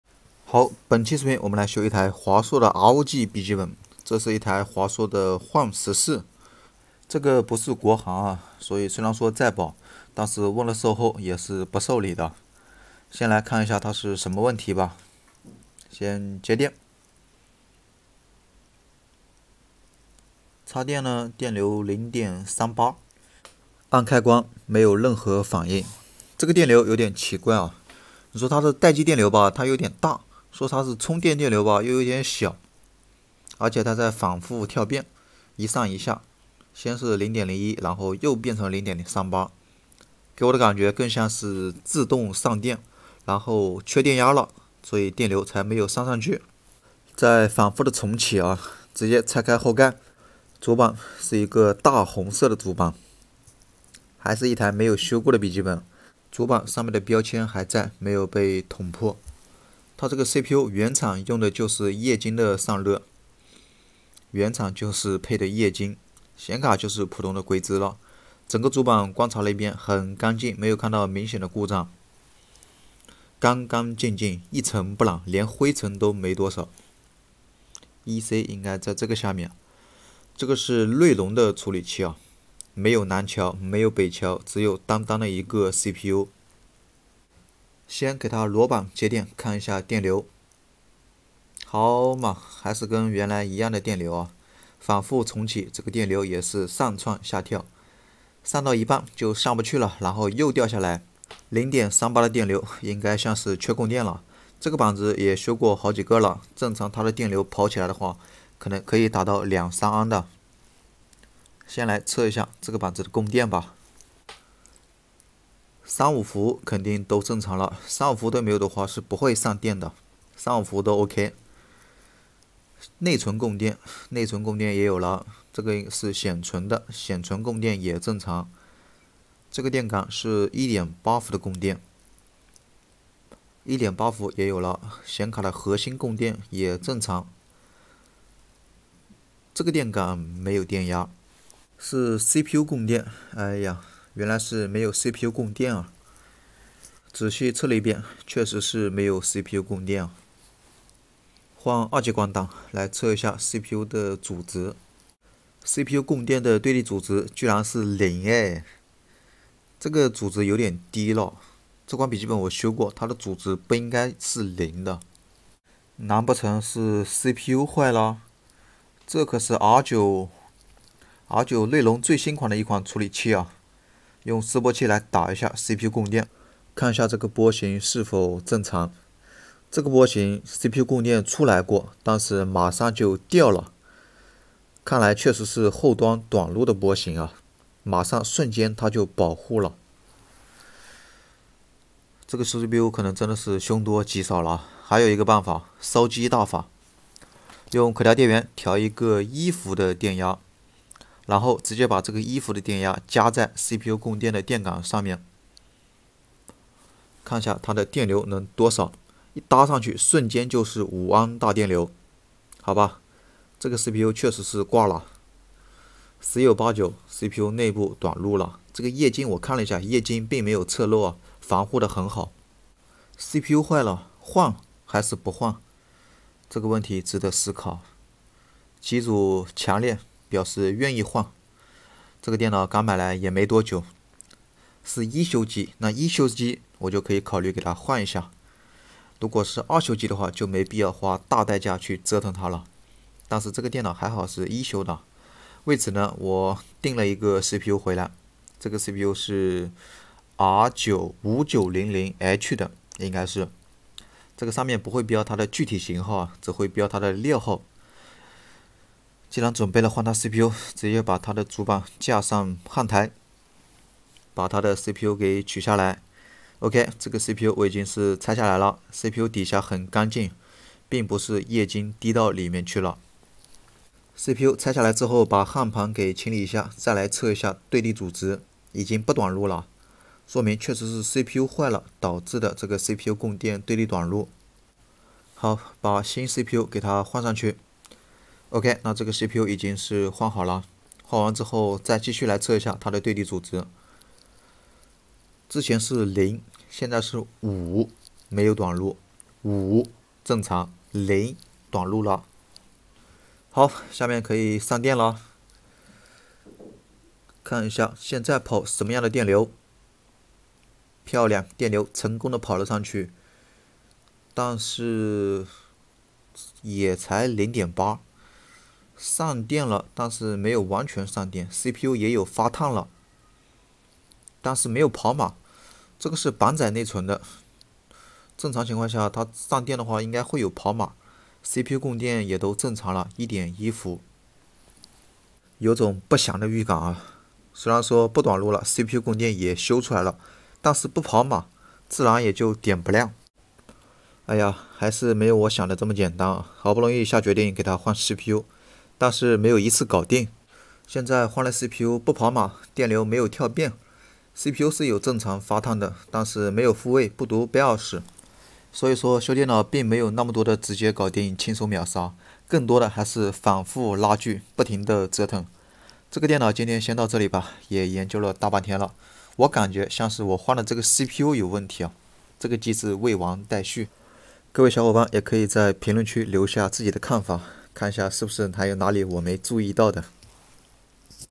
【笔记本维修】不是国行所以售后不受理！一台华硕ROG幻14换cpu失败！【靓仔修机 PC-Repair】
我们的视频是为了为观众提供娱乐，同时也保证了拍摄过程的安全。 【笔记本维修】不是国行所以售后不受理！一台华硕ROG幻14换cpu失败！【靓仔修机 PC-Repair】